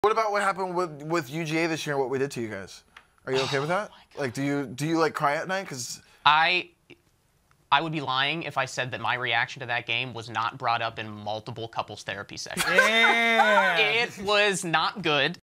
Football Ruins Relationship?!
Comedian Matt Bowman talks about how a football loss affected his relationship.